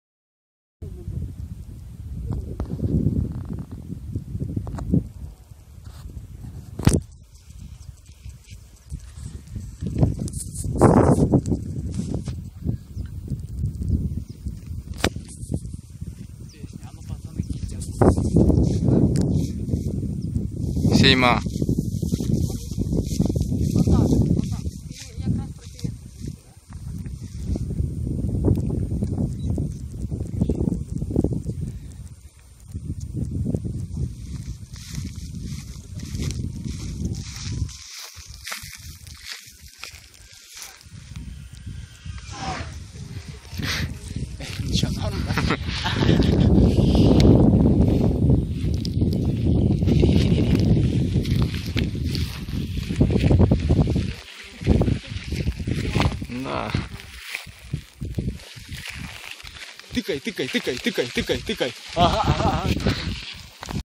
Launch Supra F3J model by Vladimirs Model glider
A small flying with friends

Pilot: Andrey Kalembet
Model: Supra 3.5M